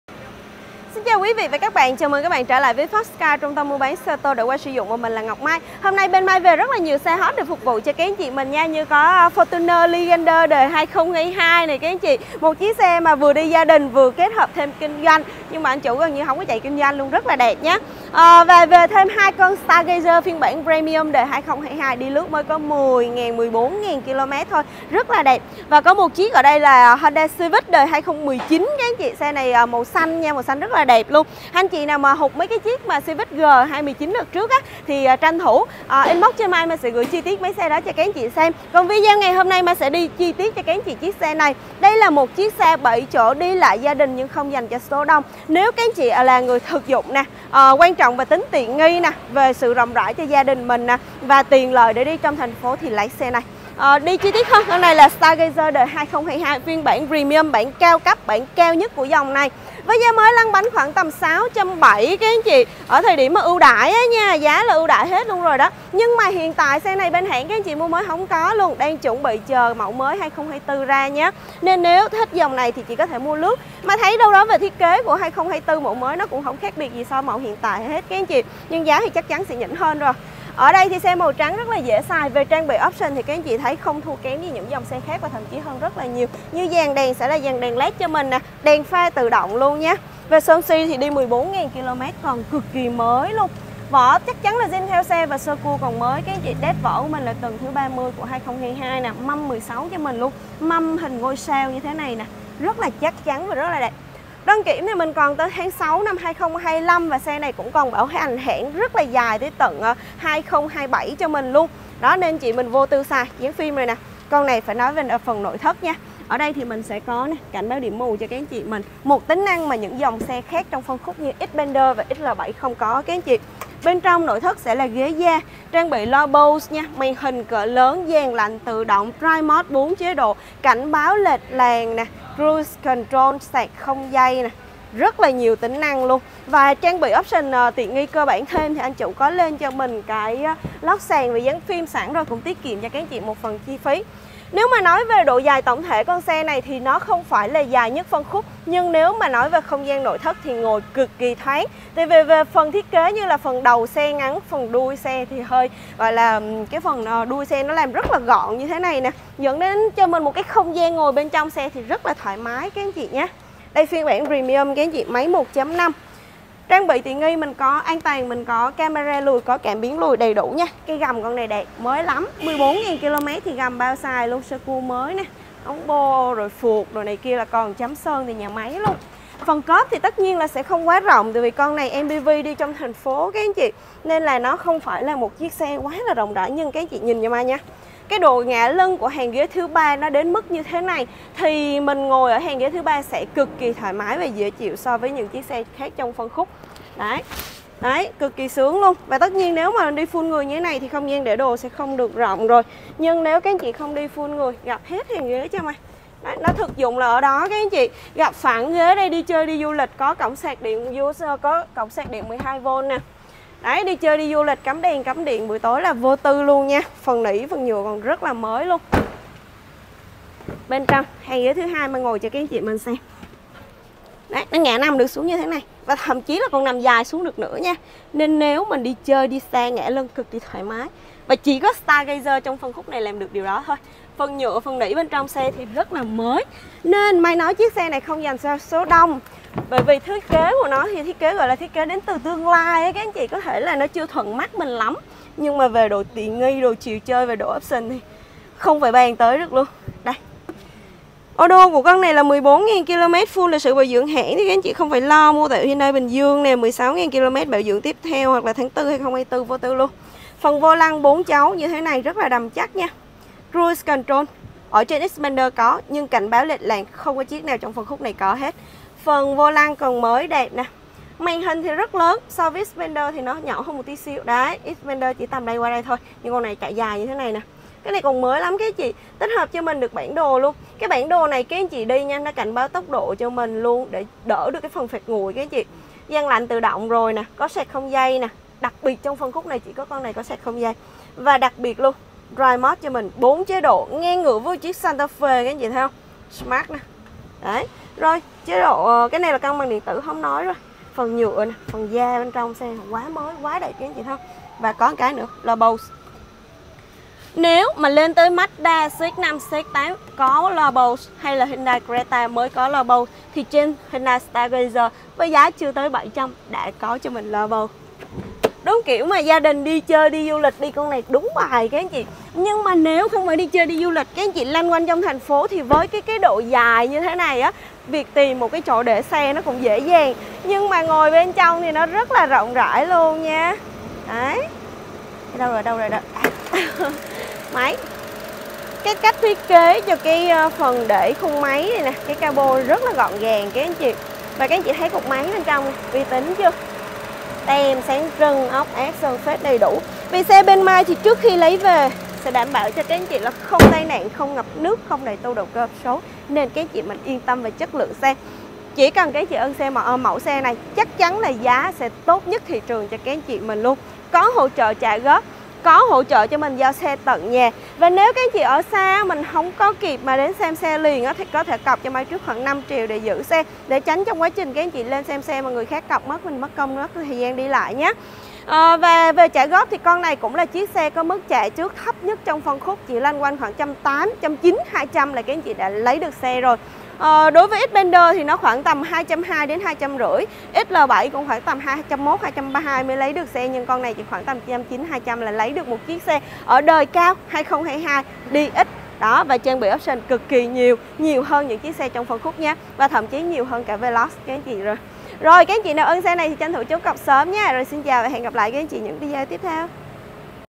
Xe 7 chỗ giá rẻ, tiện nghi 🚘 Hyundai Stargazer 2022 Premium lướt 14.000 với 200 tr nhận xe
Xe 7 chỗ giá mềm, rộng rãi, full option

🚘 Huyndai Stargazer 2022, 1.5AT Premium cao cấp
- Lướt 10.000km, bảo dưỡng hãng đầy đủ
- Trang bị sẵn: ghế da, màn hình giải trí, cảnh báo lệch làn, cruse control, đề nỗ từ xa, cảm biến de, camera lùi…
- Dàn lốp zin mới 98%, sơ cua chưa hạ
- Bảo hành hãng còn dài

- Giá chỉ 5xx triệu - có thương lượng